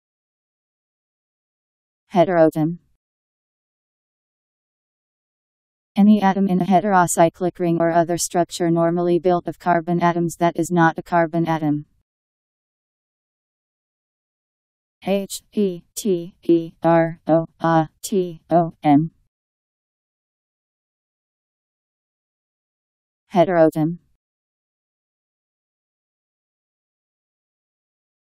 Heteroatom Meaning
Video shows what heteroatom means. Any atom in a heterocyclic ring (or other structure normally built of carbon atoms) that is not a carbon atom..  Heteroatom Meaning. How to pronounce, definition audio dictionary. How to say heteroatom. Powered by MaryTTS, Wiktionary